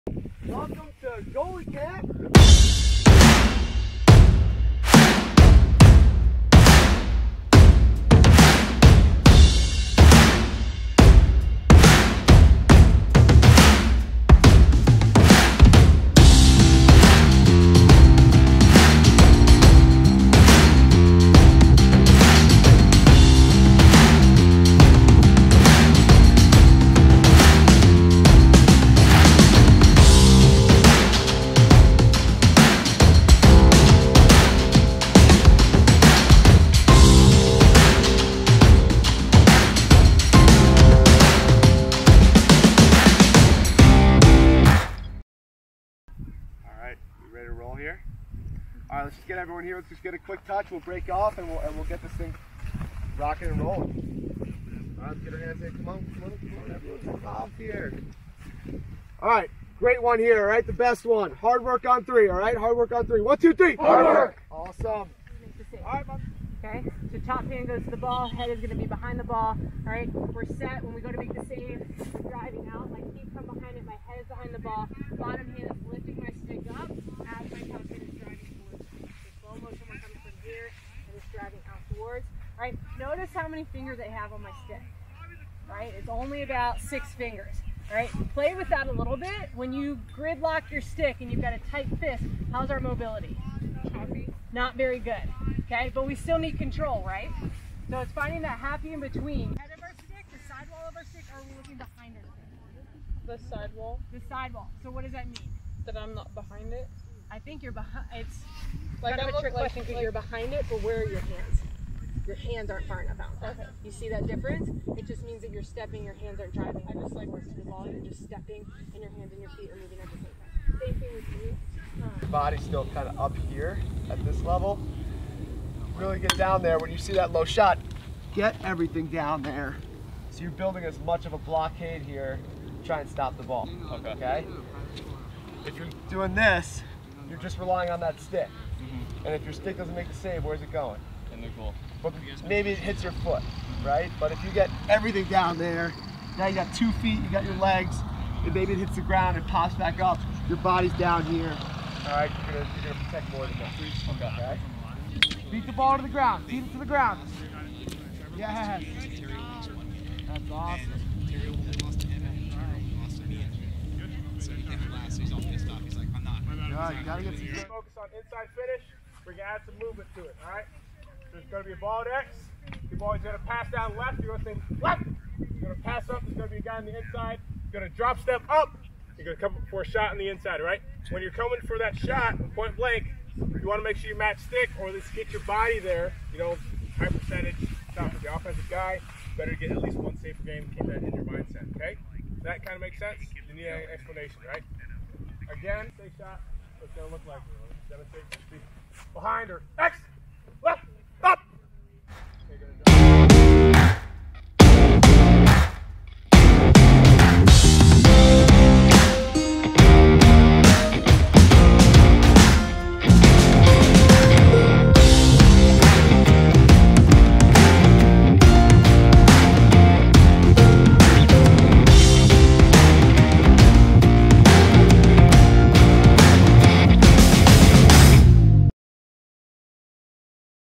G3 West Goalie Camp
Check out the behind the scenes of our G3 Goalie Camps. This video was shot at G3 West, which is located at the Olympic Training Center in San Diego. G3 Goalie camps are offered on the East Coast as well, with both camps taking place in the middle of the Summer.

for more from G3.